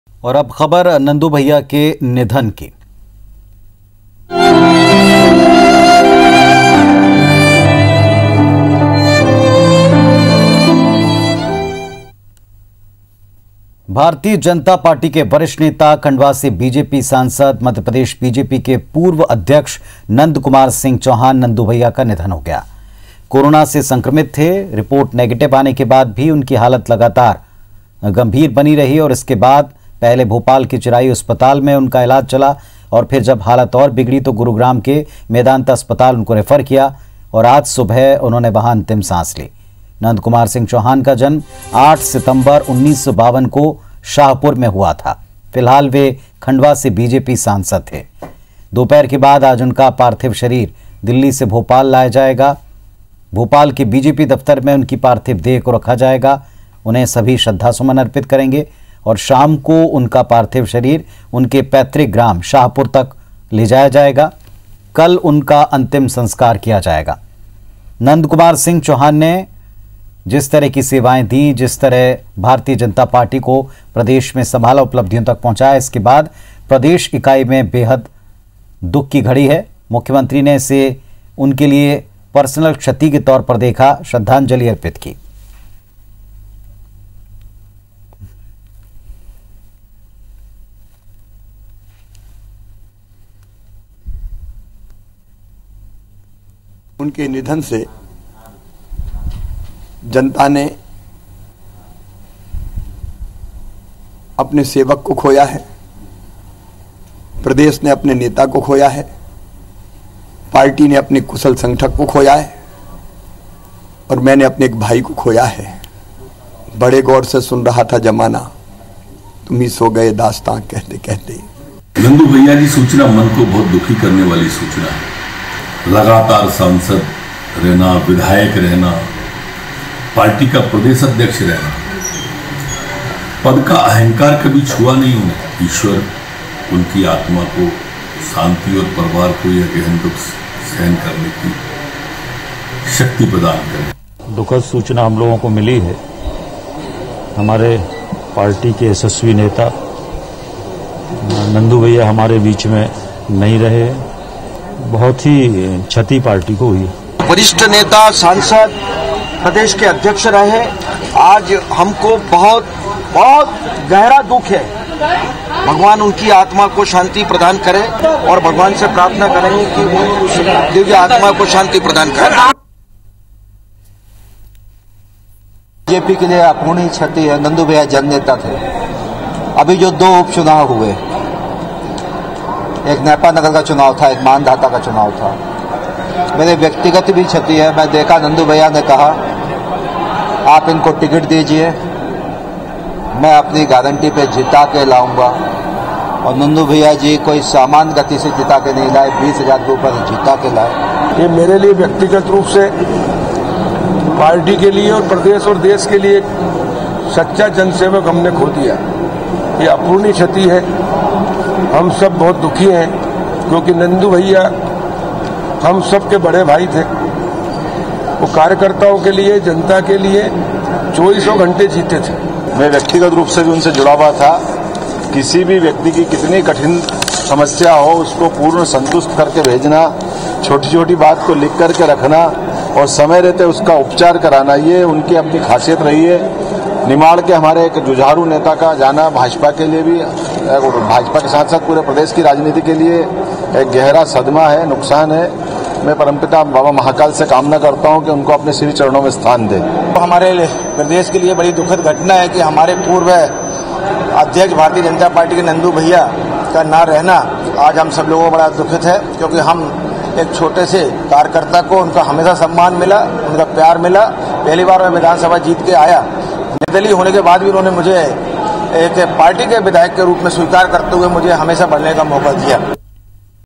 BJP सांसद Nand Kumar Singh Chauhan का निधन | जानिए ​कैसा था उनका सियासी सफर
IBC24, BJP सांसद Nand Kumar Singh Chauhan का निधन, जानिए ​कैसा था उनका सियासी सफर